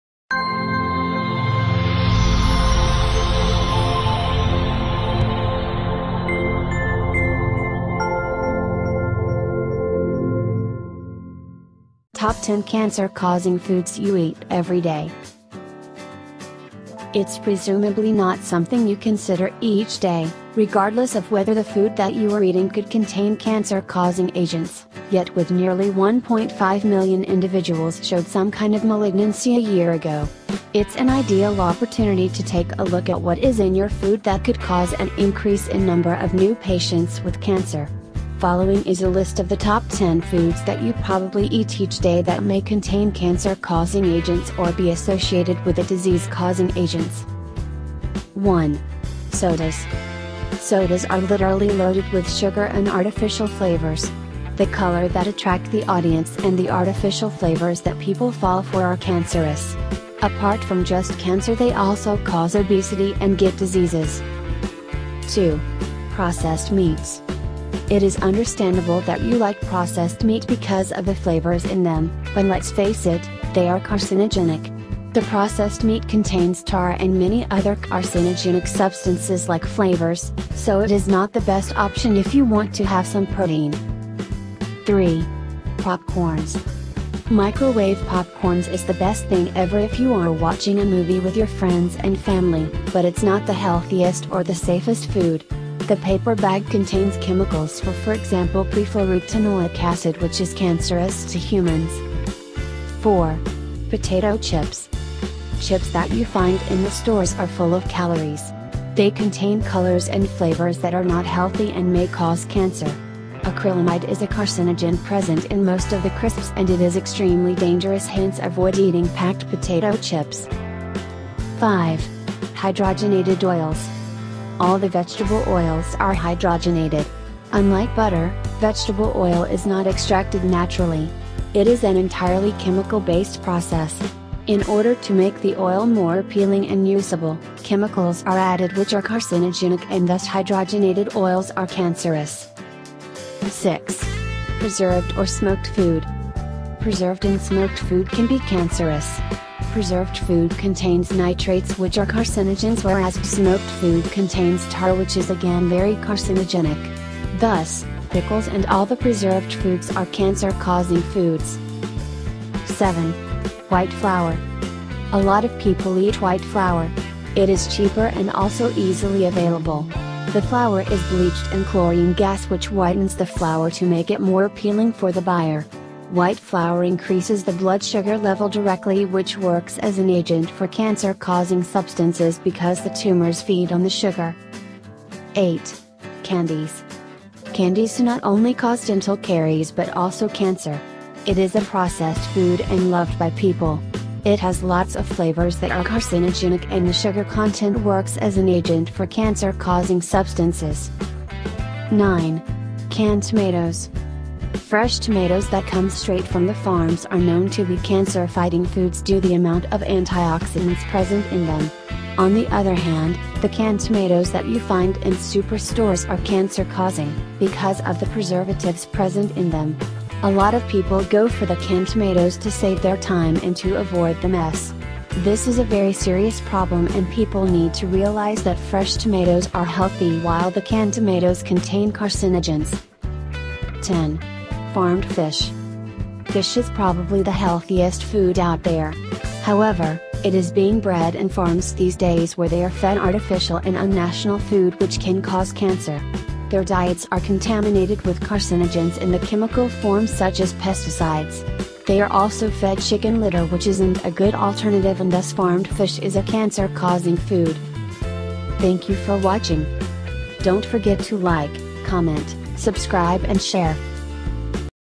Top 10 Cancer Causing Foods You Eat Every Day | Love Healthy Life
Top 10 Cancer Causing Foods You Eat Every Day

Fish is probably the healthiest food out there. However, it is being bred in farms these days where they are fed artificial and unnatural food which can cause cancer. Their diets are contaminated with carcinogens in the chemical form such as pesticides. They are also fed chicken litter which isn’t a good alternative and thus farmed fish is a cancer causing food.